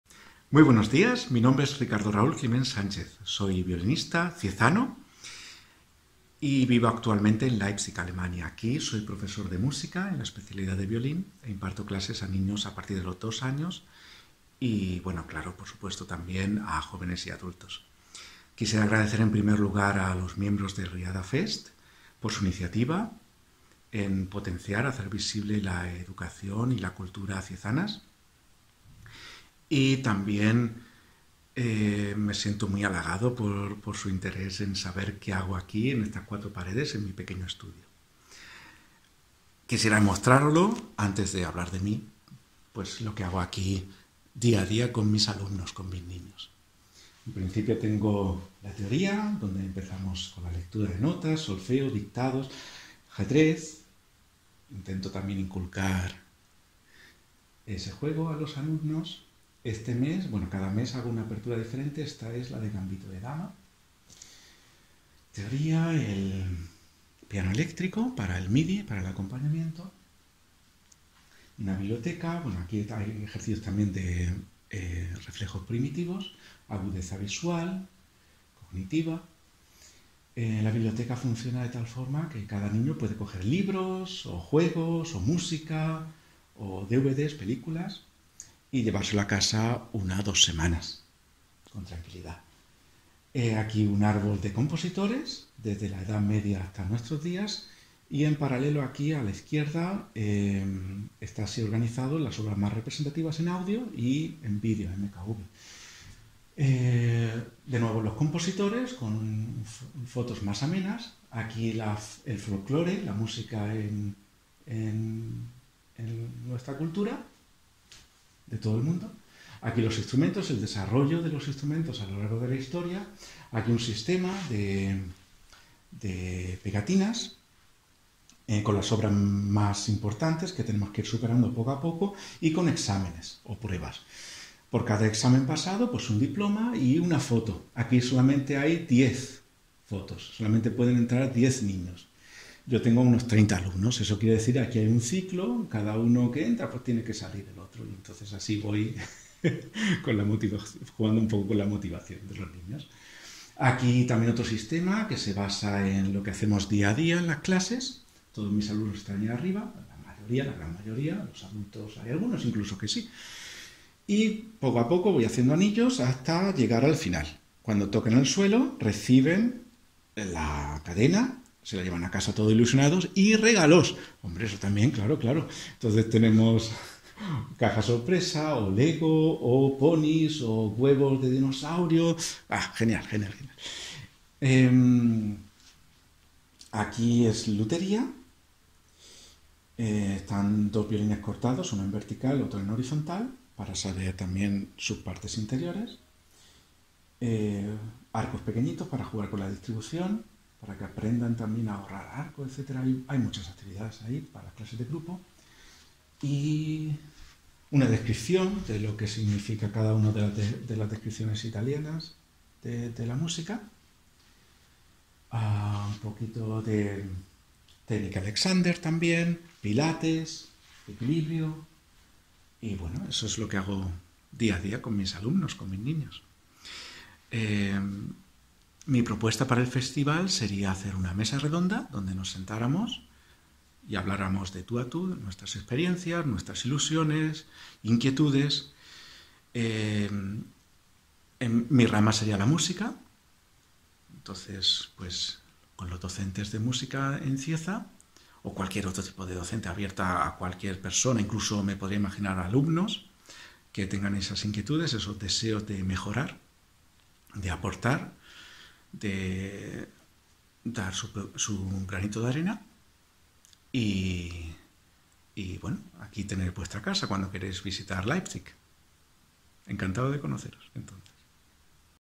Riada Fest Cieza 2020 - Raúl Climent, profesor de violín
Presentación de proyecto pedagógico "mesa redonda".